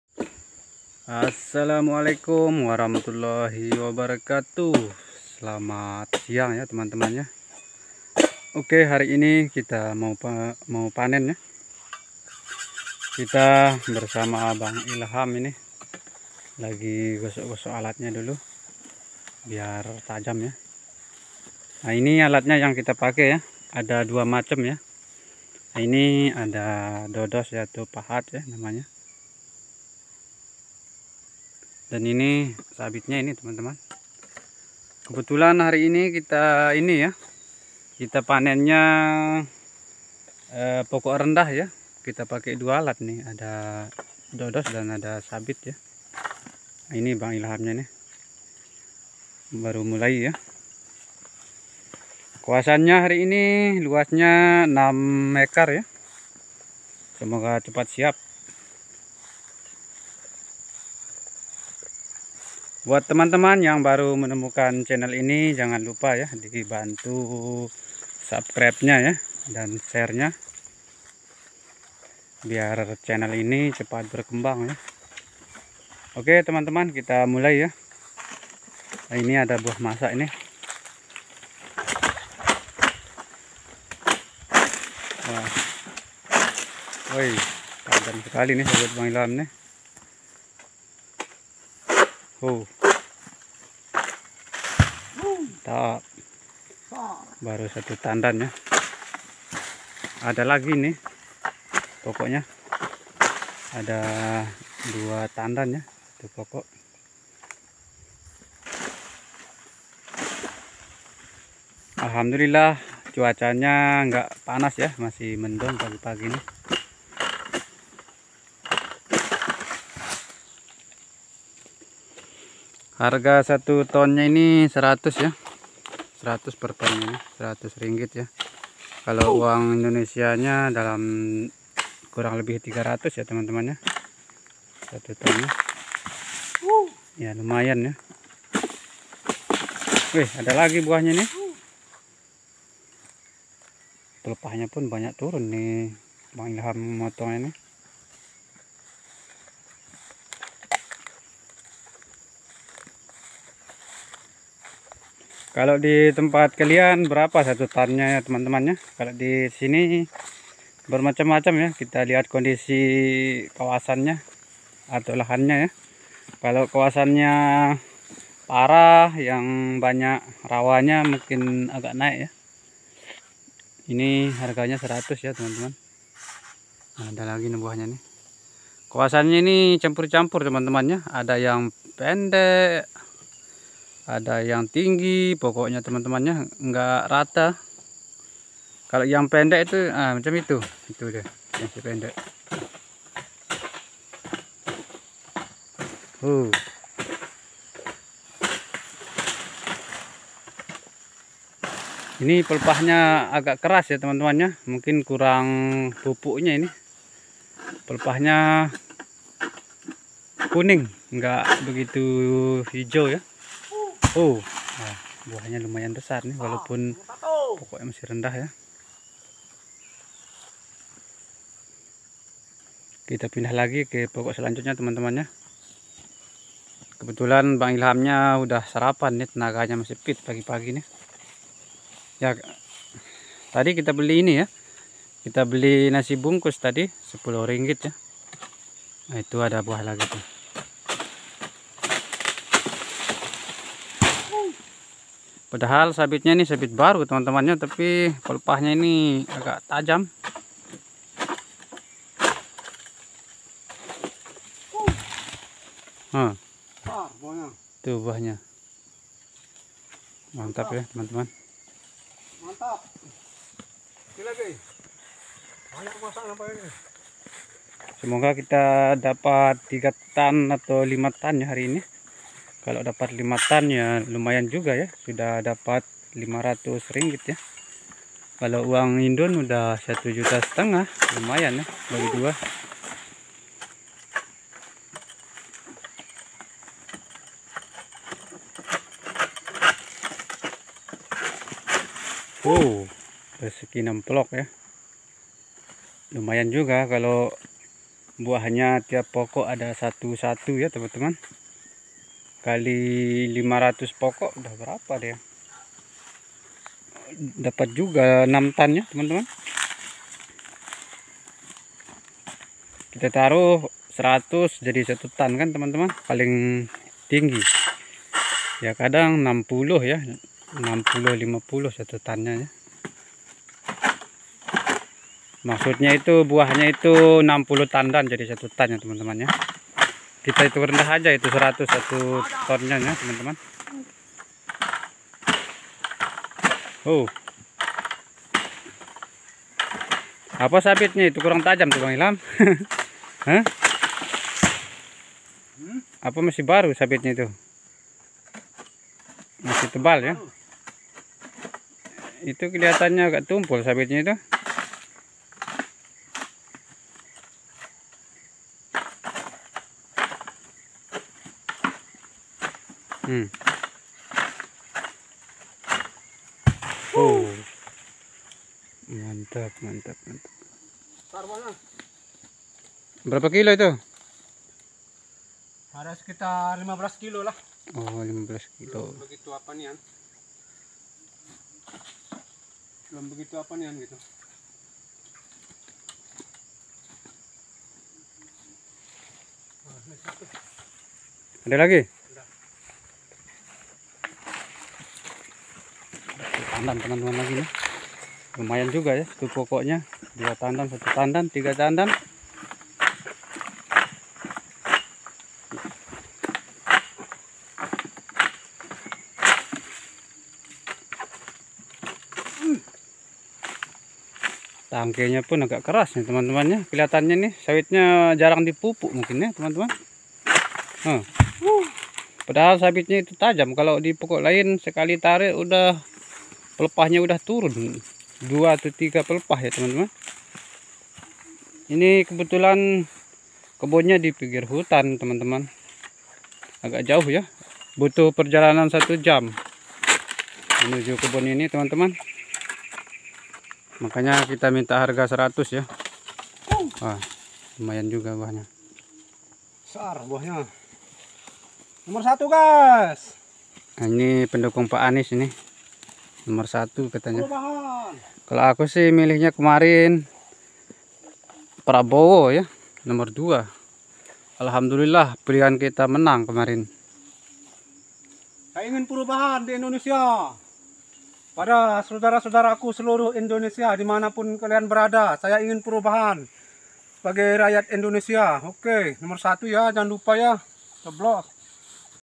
panen sawit pokok rendah, kebun tetangga jauh berbeda, panen sawit di Malaysia.
panen sawit pokok rendah, kebun tetangga jauh berbeda, panen sawit di Malaysia Alhamdulillah buah sawit ya selalu ada tiap rotasi pusingan, kebun ini luasnya 3 ekar lokasi di pinggir hutan.